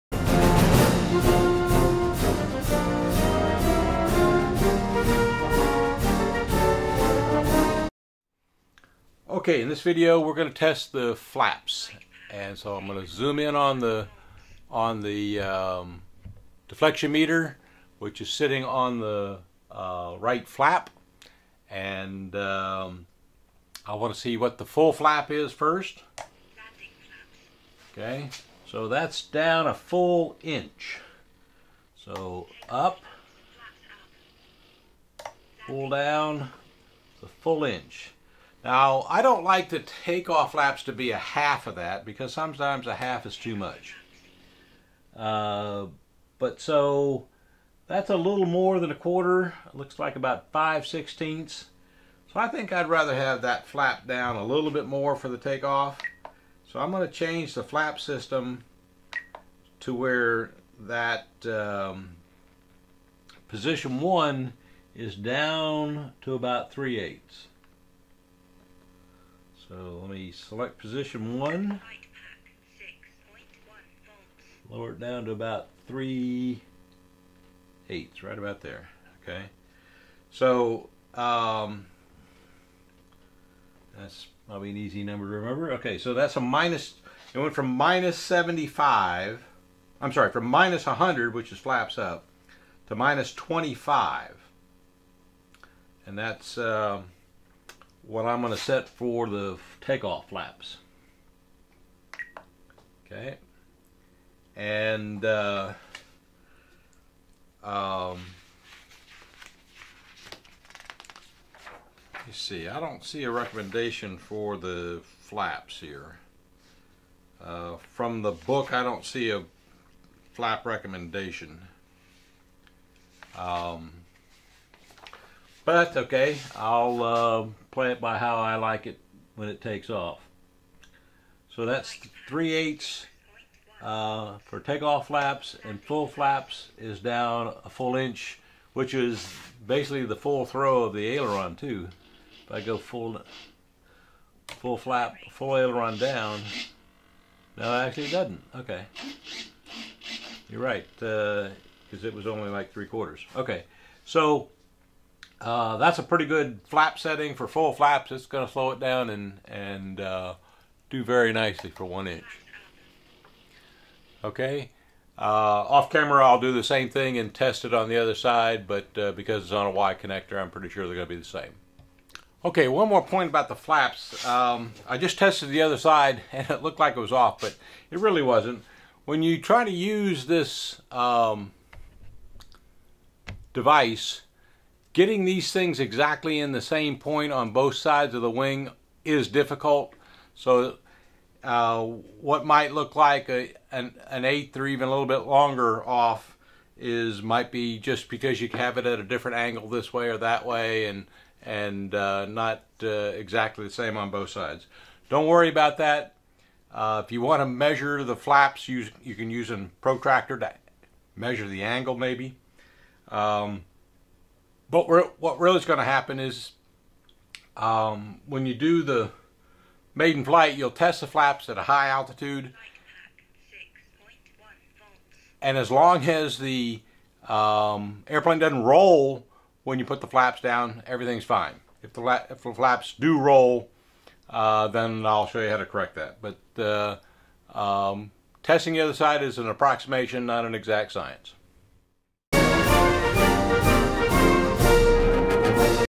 AV50 Avistar Flap Control Throw
This video shows how to set the control throw for the flaps of the Great Planes Avistar nitro powered RC aircraft.